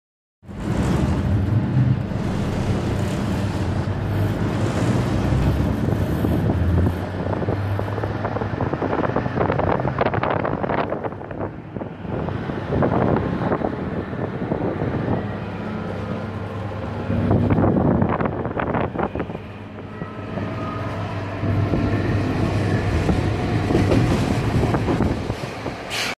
[Full HD] BDVmot gyorsulás - 414 007 2017.08.10
H-Start Ganz-MÁVAG BDV Hernyó típusú villamos motorvonat indul Mezőkeresztes-Mezőnyárád állomásról Füzesabonyba 5051-es Miskolcról induló személyvonatként.